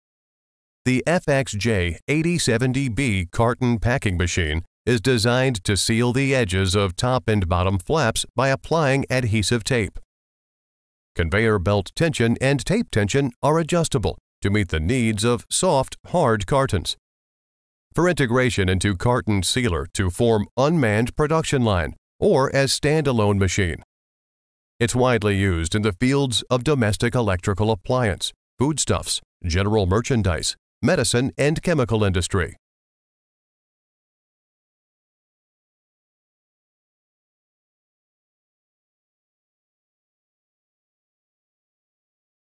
FXJ 8070B Semiautomatic Carton Sealer (เครื่องปิดกล่องเทปกาวกึ่งอัตโนมัติสำหรับกล่องใหญ่)
ซีรีส์ JFX ได้รับการออกแบบมาเพื่อปิดผนึกขอบของแผ่นพับด้านบนและด้านล่างโดยใช้เทปกาว ความตึงของสายพานลำเลียงและความตึงของเทปสามารถปรับได้เพื่อตอบสนองความต้องการของกล่องอ่อน/แข็ง สำหรับบูรณาการเข้ากับเครื่องปิดผนึกกล่องเพื่อสร้างสายการผลิตแบบไร้คนควบคุมหรือเป็นเครื่องจักรแบบสแตนด์อโลน การเปลี่ยนขนาดได้ง่ายเหมาะอย่างยิ่งสำหรับการใช้งานที่ต้องการการป้องกันการรั่วไหลในระดับสูง

คุณสมบัติ:

√ ประสิทธิภาพที่เชื่อถือได้

√ ใช้งานง่าย

√คุ้มค่า

√ความเร็วสูง

√ เปลี่ยนขนาดได้ง่าย

√กลไกการตั้งศูนย์กลางรางนำ

√สายพานขับสองตัวบนและล่าง

√ มีจำหน่ายสำหรับสายการผลิต

แอปพลิเคชัน:

1. ซีรีส์ JFX ได้รับการออกแบบมาเพื่อปิดผนึกกล่องโดยใช้เทปกาว

2. มีการใช้กันอย่างแพร่หลายในด้านเครื่องใช้ไฟฟ้าภายในบ้าน อาหาร สินค้าทั่วไป ยา และอุตสาหกรรมเคมี

3.เพื่อใช้ในสถานที่ที่มีเนื้อที่เพียงพอ เช่น โรงงาน และโกดังสินค้า เป็นต้น